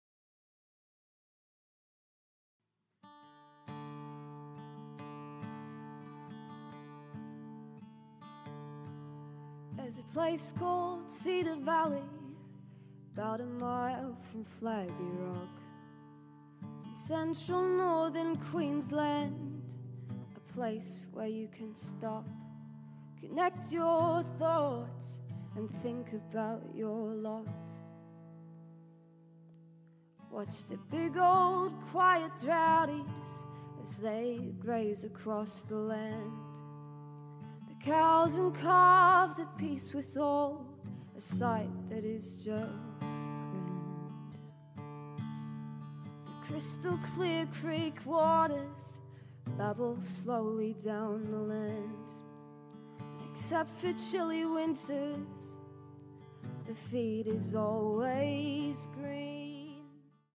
Ceda Valley
Nutrien Harcourts Present 'Ceda Valley' to Auction
Thursday 17th September, 2020
Souths Leagues Club, Mackay @ 11.00am